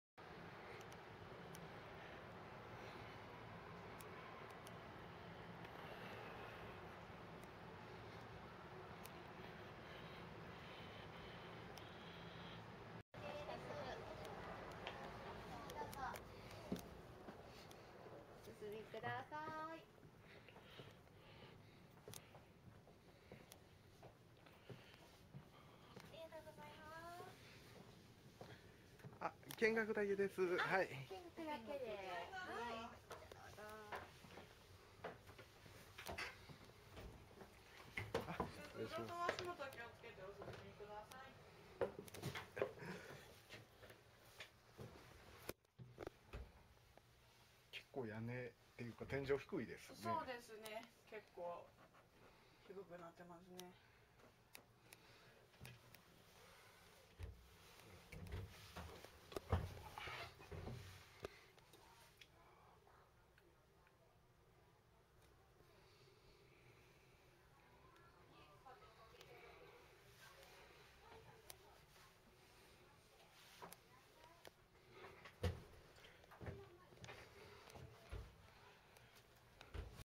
100 Series Shinkansen motorman's cab 100系新幹線運転台・運転席
鉄道趣味を楽しんで「好きなことで、生きていく」方法

Japan Railway Movie
JR Central Japan
SCMaglev And Railway Park (Museum)
Nagoya
100 Series Shinkansen motorman's cab

JR東海のリニア・鉄道館に展示されている
100系新幹線の運転台・運転席